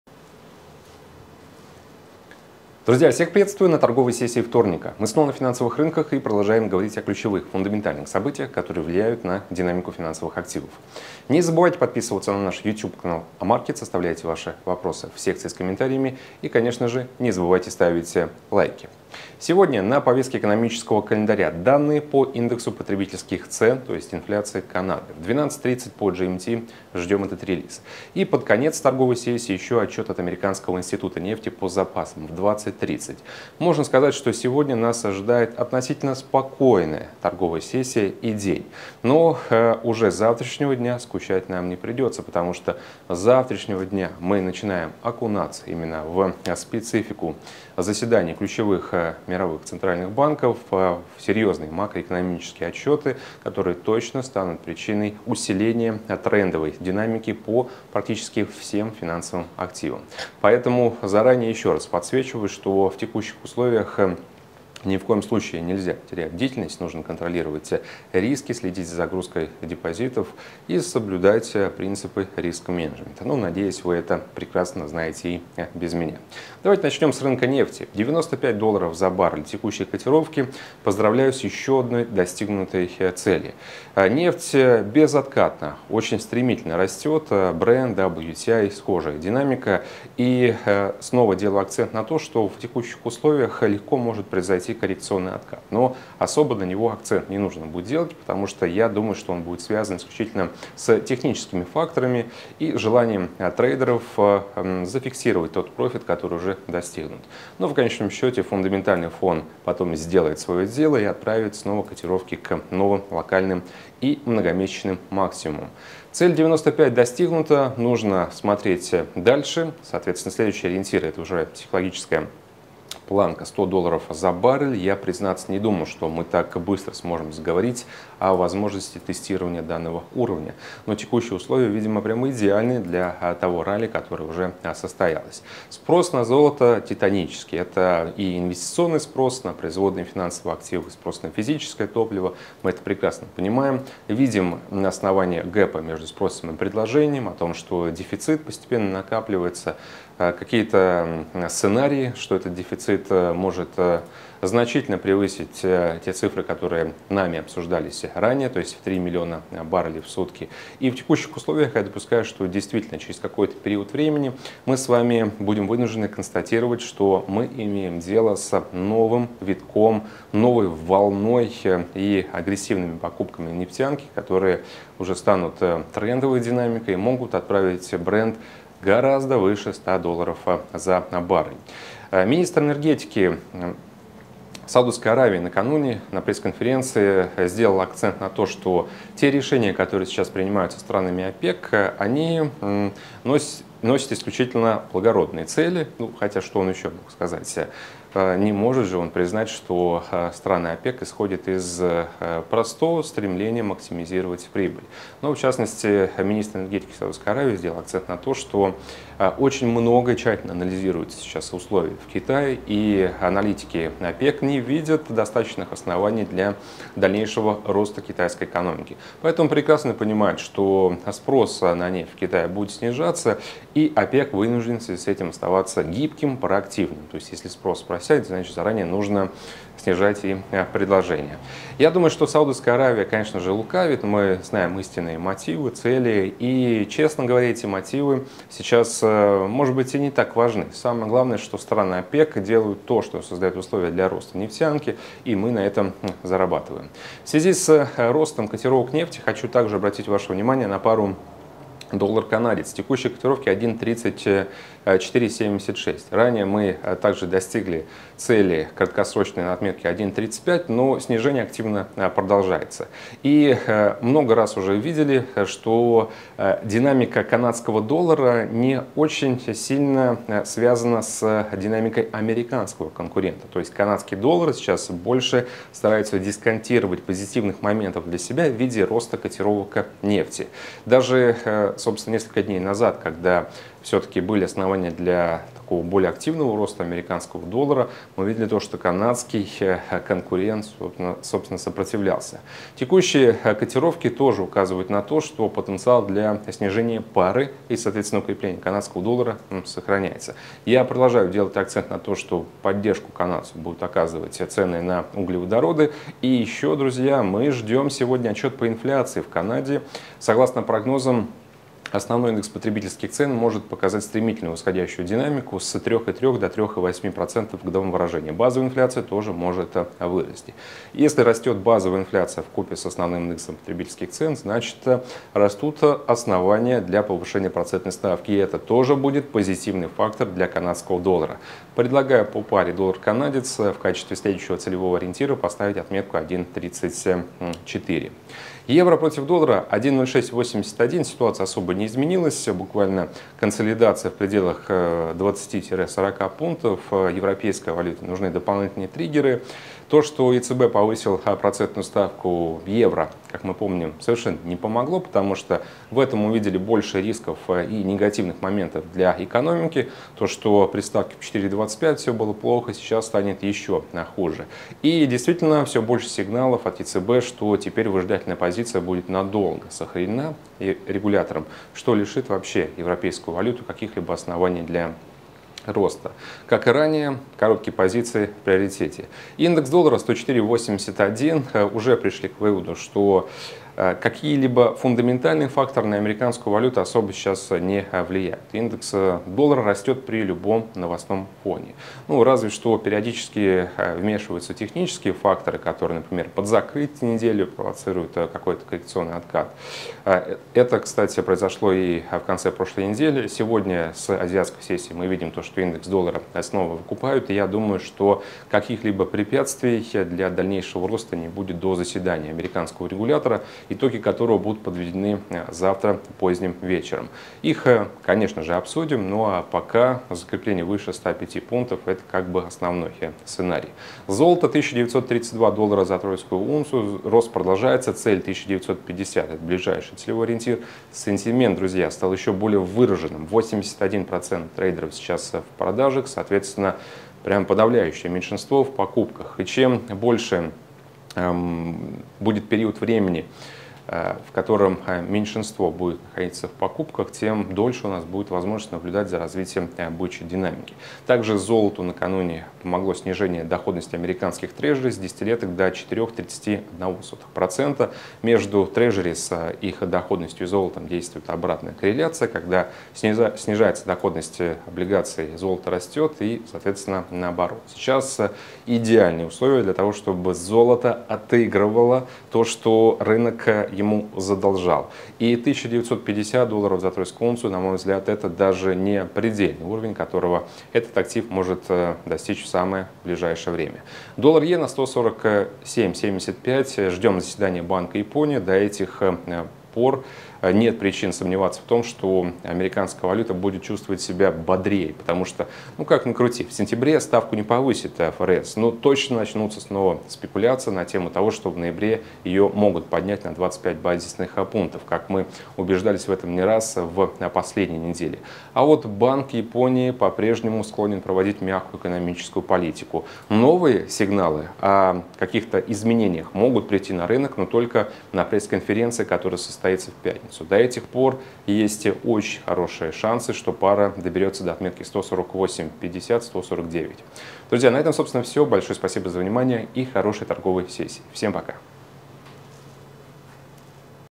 Будет ли доллар расти? | AMarkets
Будет ли доллар расти, что решит Федеральная Резервная система на заседании в среду, почему евро будет снижаться, что поддерживает рынок драгоценных металлов, как заработать на паре USD/CAD, о перспективах пары USD/JPY, достигнет ли Brent $100 за баррель